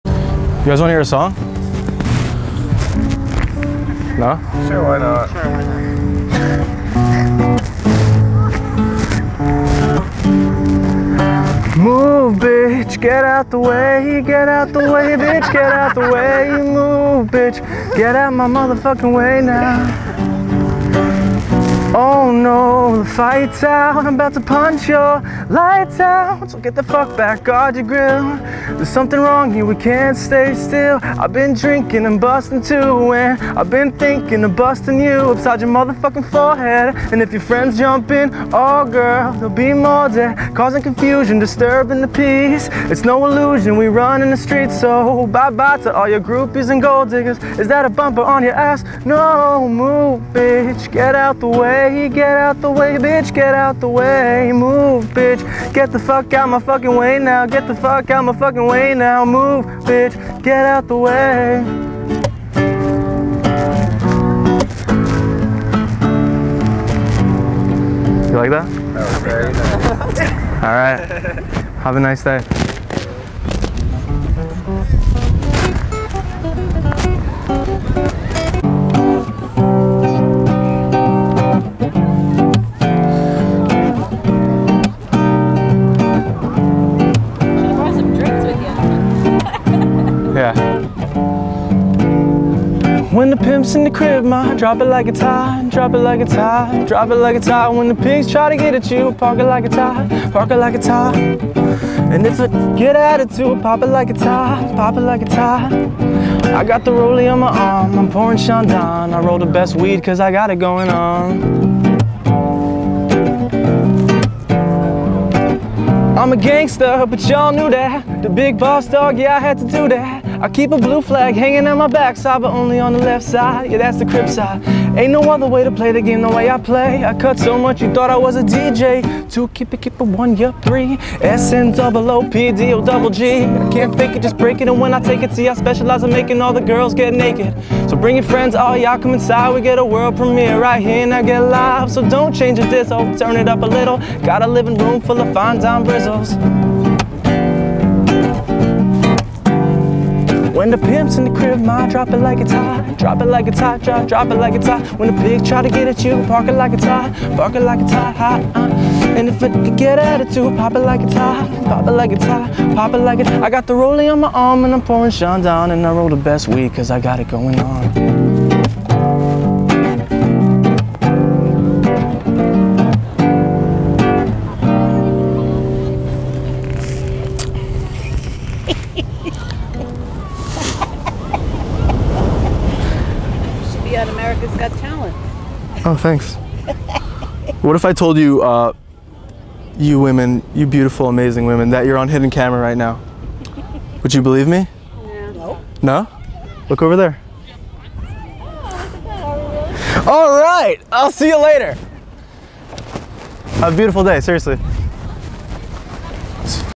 Serenading Old People with Snoop Dogg Songs - Zalman Krause
Hey guys - here are some extra serenades from last week's episode. Hope ya'll enjoy.